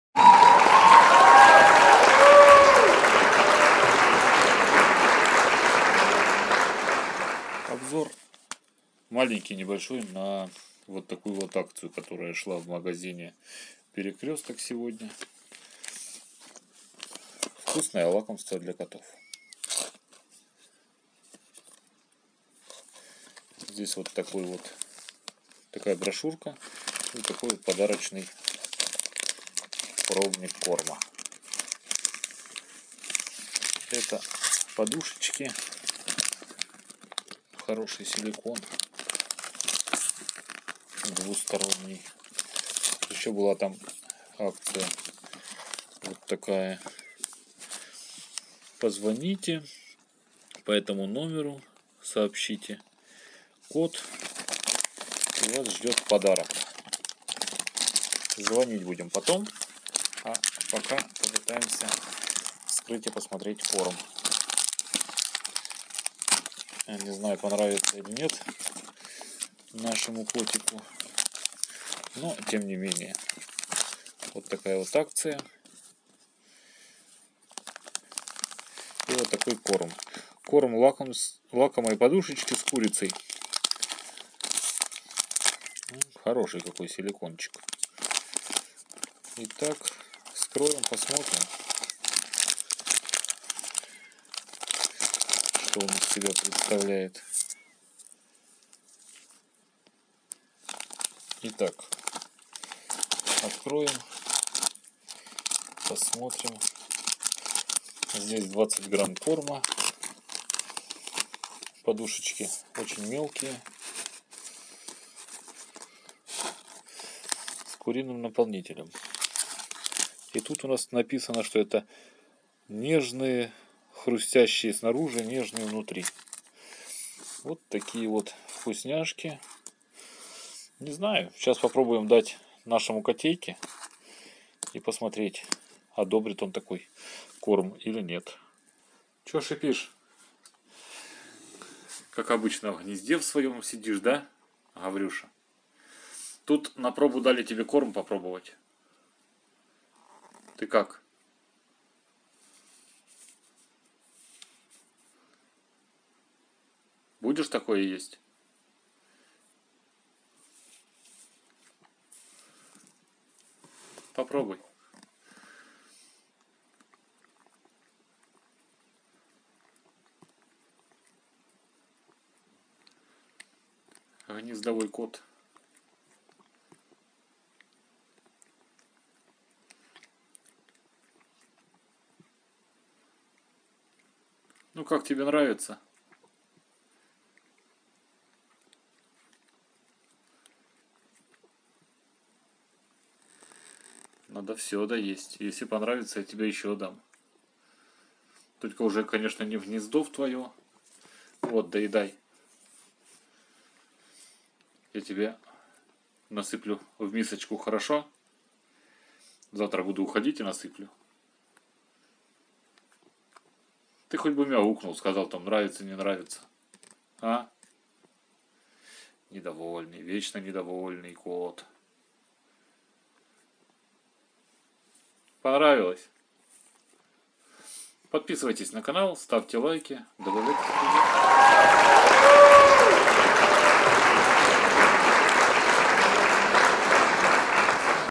Корм DREAMIES вкусное лакомство для котов
Dreamies™ – это лакомство, хрустящее снаружи и невероятно нежное внутри.
Угостили своего кота лакомыми подушечками Dreamies, когда нам в качестве раекламы вручили промо-упаковку. С тех пор ежедневно угощаем своего пушистого любимца лакомыми подушечками Dreamies в количестве не более 20 штук - утром около 10 штук...

Мои Рецепты
Рецепты на каждый день